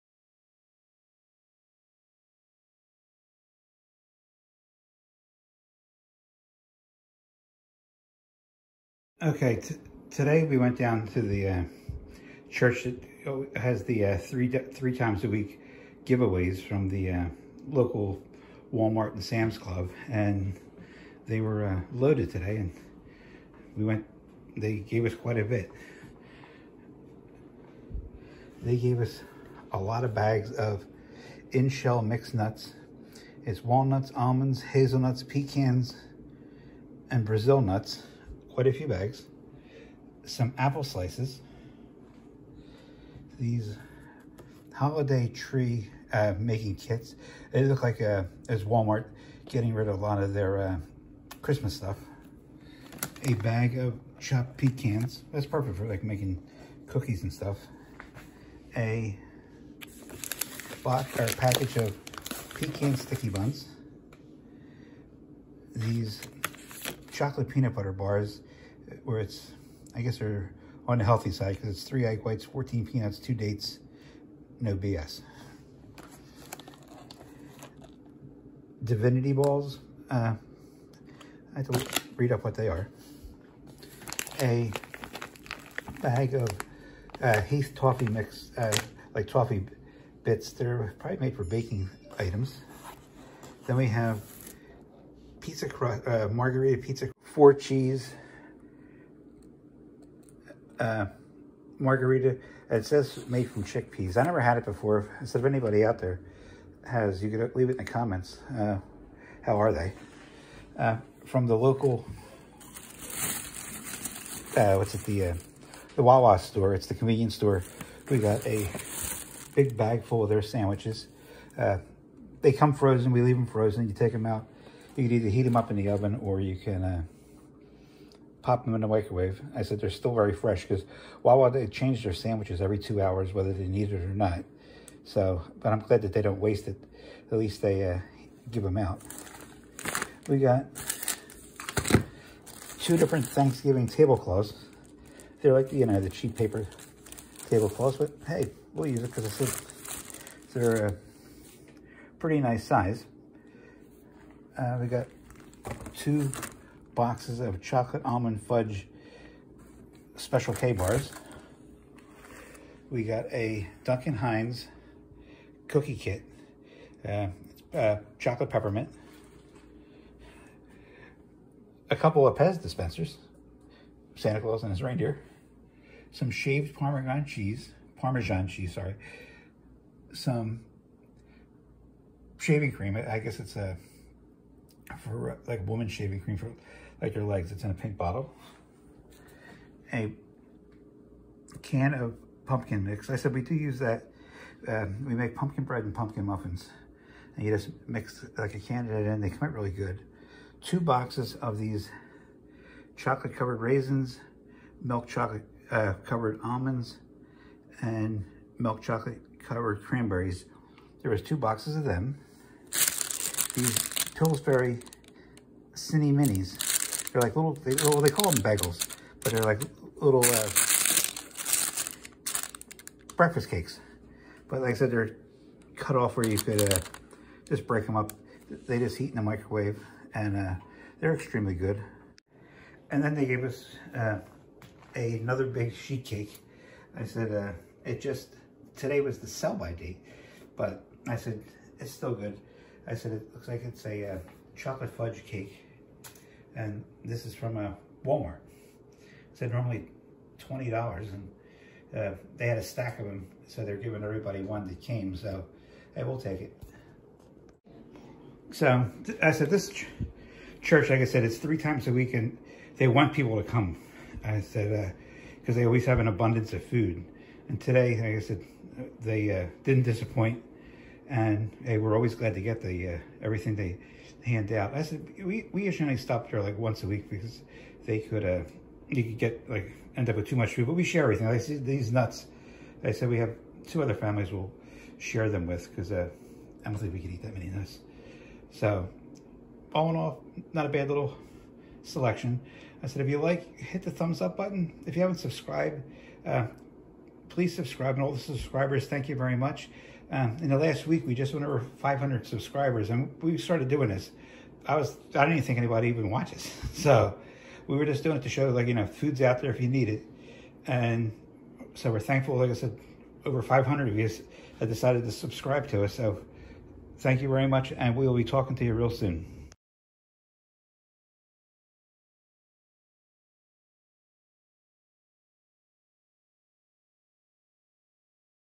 Unexpected Trip To The Food Pantry Nice Haul
The local pantry church had a lot of food to give out so we went